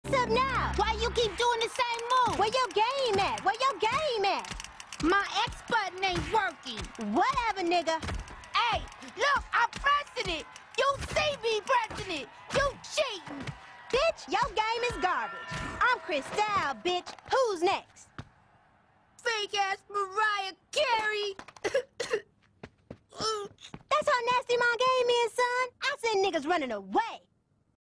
Where Yo Game At?
naruto (Voiced by Riley ) plays a game against lady tsunade (Voiced By The Garden Tool)